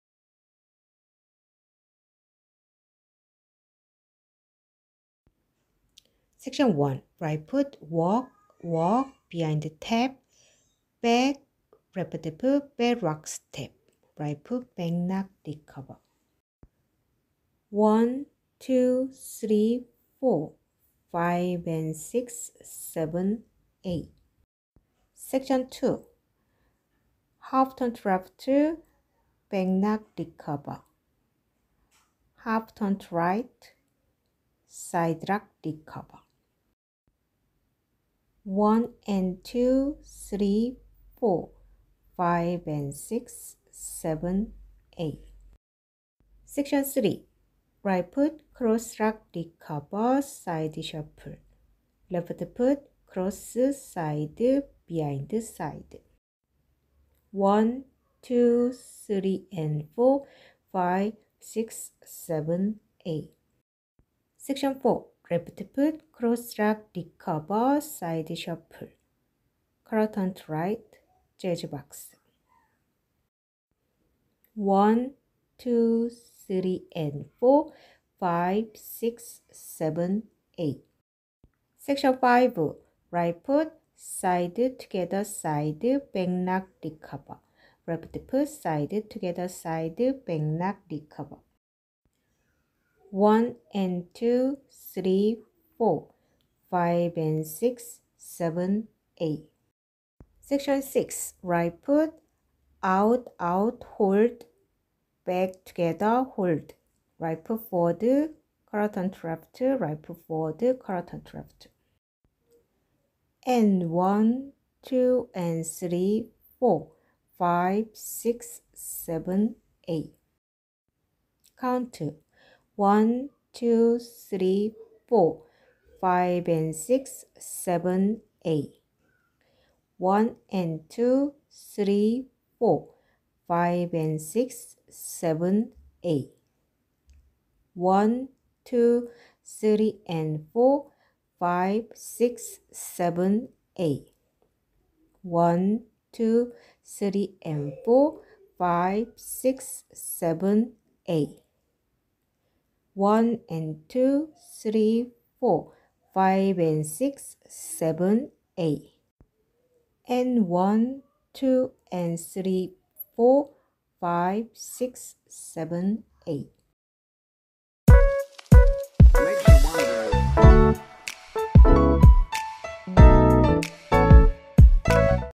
라인댄스 Tutorial | Cowboy Style linedance | Beginner | 카우보이 스타일 초급 라인댄스 스텝
💟Cowboy Style
Count:48
Wall:4
Level:  Beginner
No Tag and  No Restart
Choreo:Ira Weisburd (USA) - October 2022